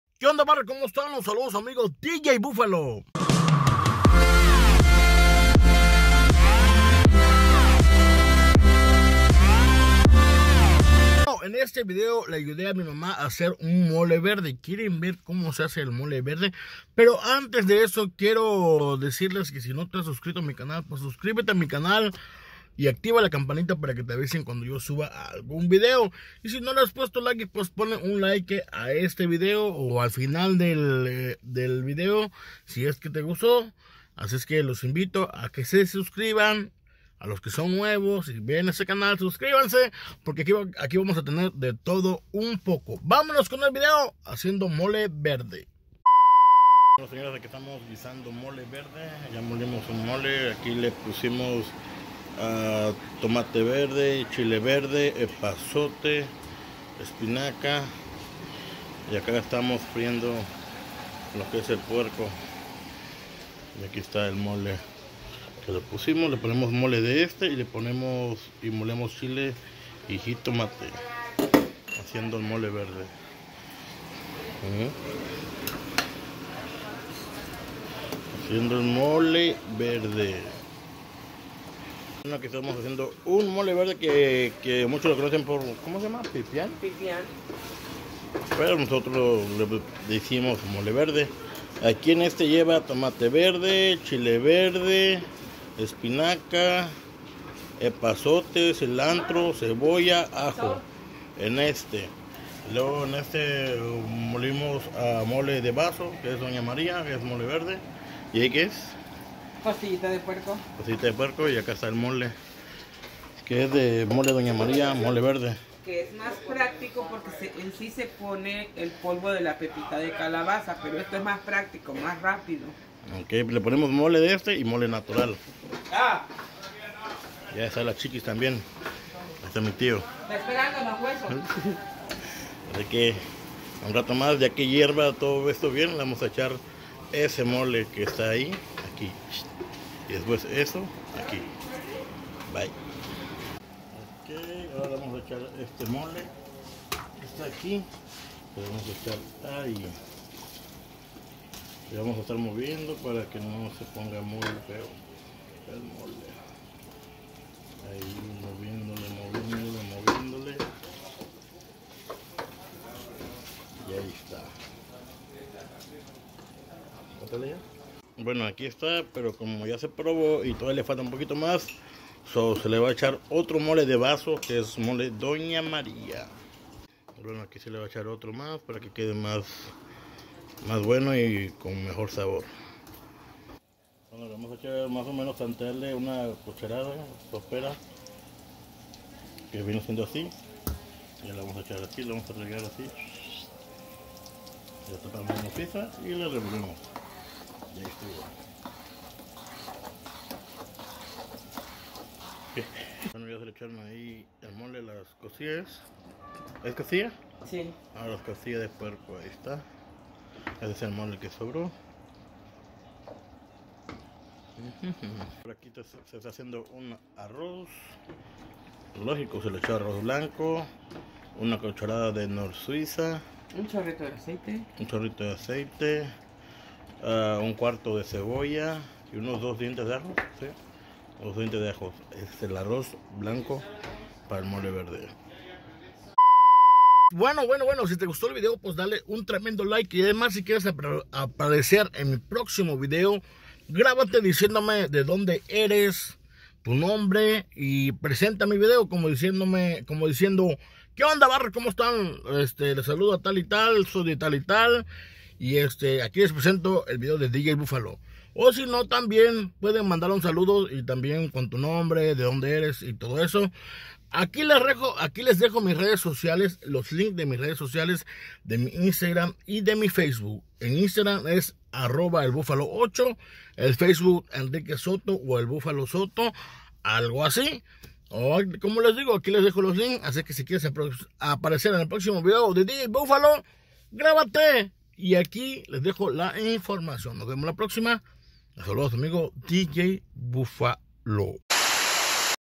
Haciendo Mole Verde
Espero Y Les Guste Este Pequeño Video Ayudando A Mi Mama Haciendo Mole Verde...

Si Quieres Aparecer En El Proximo Video...
Grabate Diciendo Como Te Llamas.. De Donde Eres.. Presenta Mi Video o Manda Saludos...